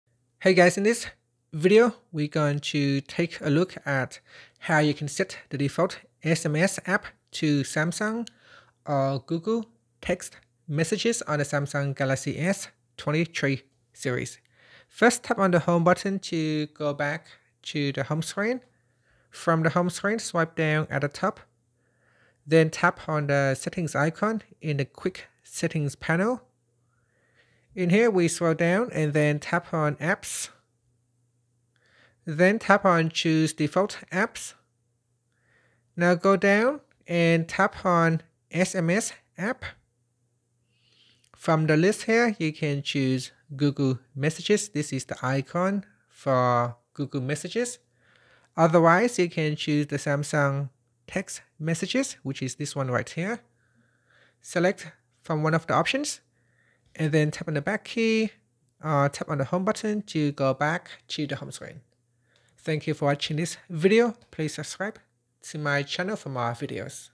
Galaxy S23's: How to Set the Default SMS App to Samsung/Google Text Messages
Learn how you can set the default SMS app to Samsung Text messages or Google Text messages on the Samsung Galaxy S23/S23+/Ultra.

In this comprehensive tutorial, we're diving into the world of Samsung Galaxy S23 and its messaging capabilities. Are you tired of searching for the right messaging app every time you want to send an SMS? Look no further, as we guide you through the process of setting the default SMS app to either Samsung Text Messages or Google Text Messages on your Samsung Galaxy S23.

Video Highlights:

- Learn how to seamlessly switch between Samsung and Google Text Messages as your default SMS app.
- Discover the advantages of having a dedicated default messaging app that suits your preferences.
- Step-by-step instructions to ensure a hassle-free experience in customizing your messaging setup.

Benefits:
Setting the default SMS app on your Samsung Galaxy S23 has several benefits:

1. Effortless Messaging: No more confusion about which app to use. Enjoy sending SMS with ease using your preferred messaging app.

2. Customized Experience: Tailor your messaging environment to your liking. Choose the app that aligns with your style and functionality preferences.

3. Enhanced Features: Both Samsung and Google Text Messages offer unique features. Select the one that complements your communication needs, whether it's rich media sharing or advanced search capabilities.

4. Seamless Integration: The chosen default app seamlessly integrates with the rest of your device, ensuring a consistent experience across your smartphone.

5. Time-Saving: With the default SMS app set to your liking, you'll save time by avoiding unnecessary app-switching when texting friends and family.

6. Personalization: Set your messaging app as an extension of your personality. Enjoy themes, color options, and other customization features that make your texting experience truly yours.